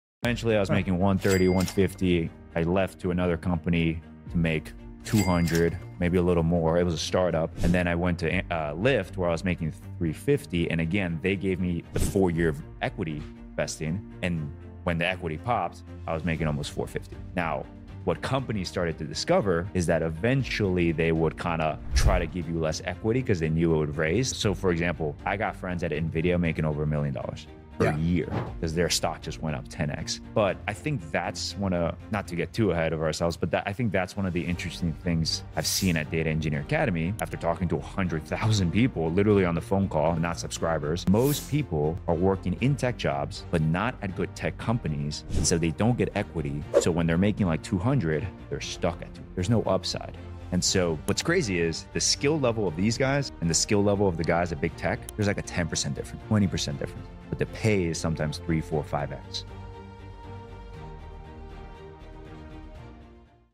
How Much Money Do Data Engineers Make?
If you want more information on the Data Engineer Academy, go

How I went from $60k to $450k in 2 years from Data Engineering. Someones there is a 10-20% skill gap that could result in a 3-5x Pay increase in the Data field